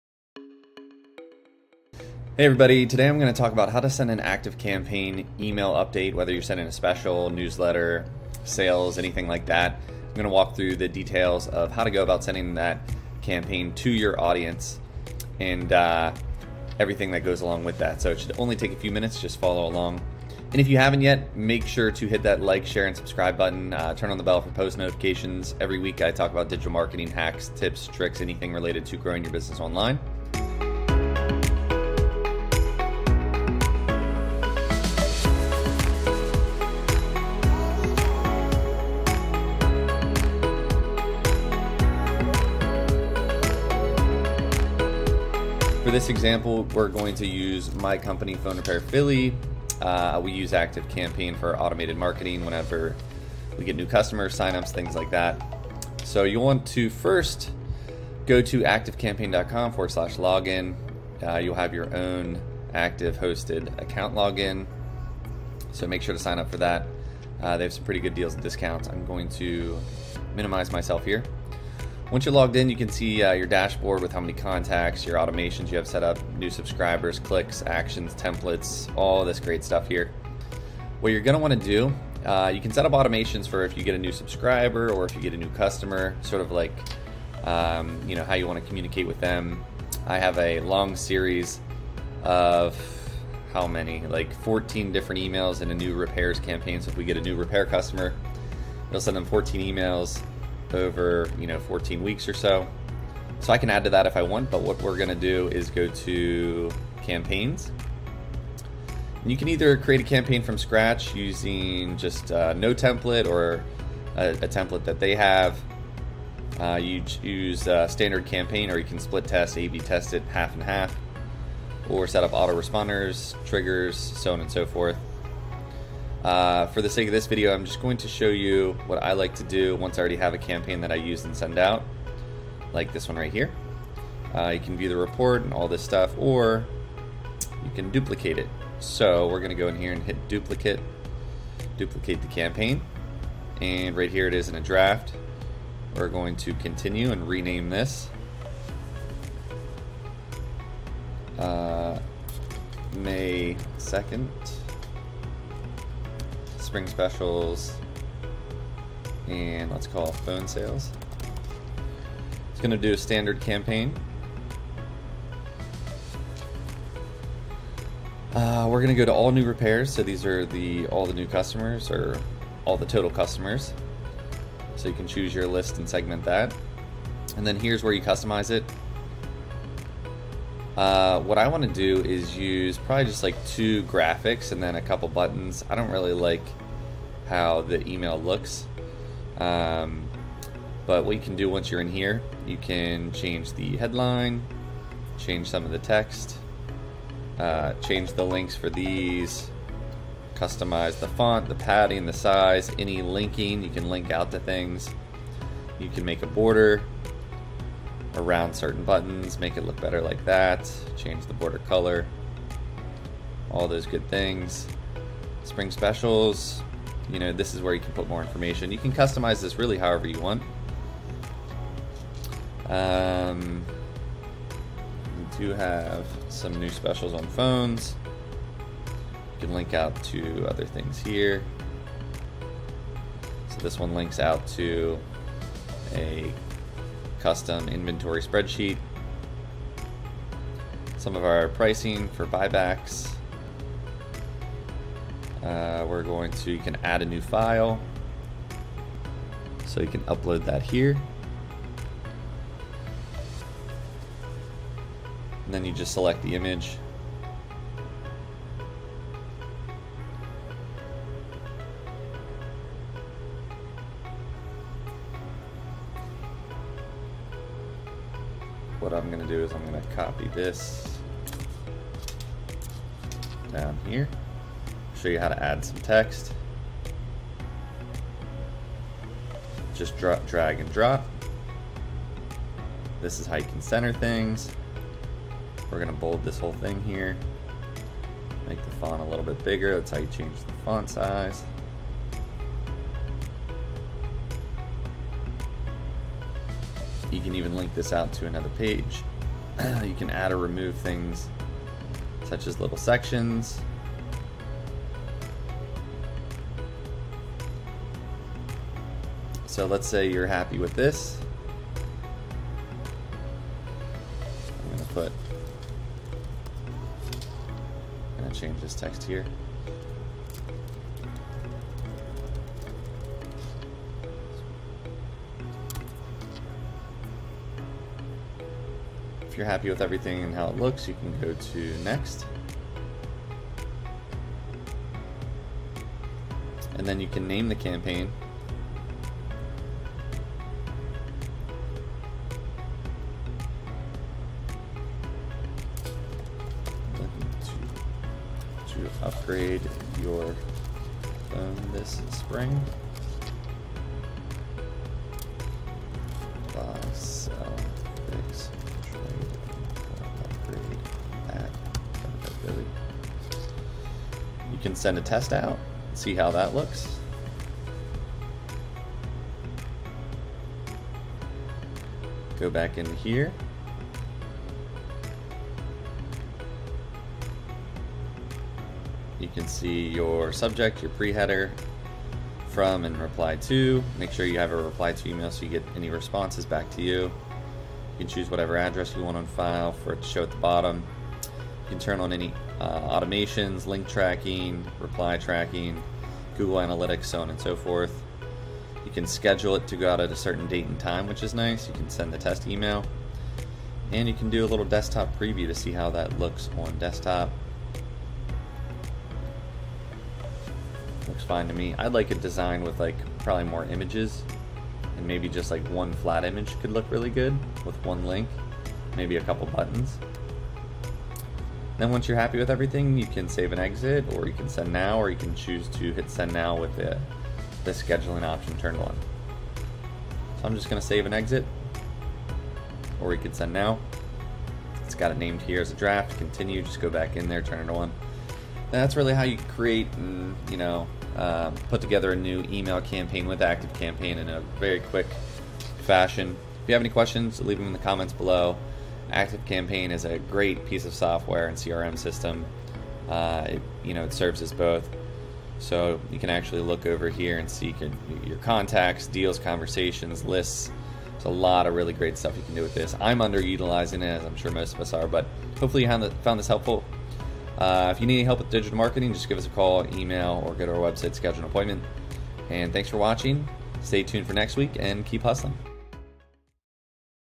How to Create New Email Campaign with ActiveCampaign
Today you will Learn How to Create New Email Campaign with ActiveCampaign.

My name is Mac Frederick, Founder & CEO of Momentum Digital.

Steps:

1. Sign up for ActiveCampaign

2. Input billing and all settings to setup ActiveCampaign account

3. Setup and connect email account to send emails

4. Create an email campaign using a template

5. Customize your email campaign
- Subject Line and Descriptions

6. Add content
- Images, text, links, colors, etc

7. Choose receiver list
- Upload or connect manual list

8. Review and Send
- Choose time and approve email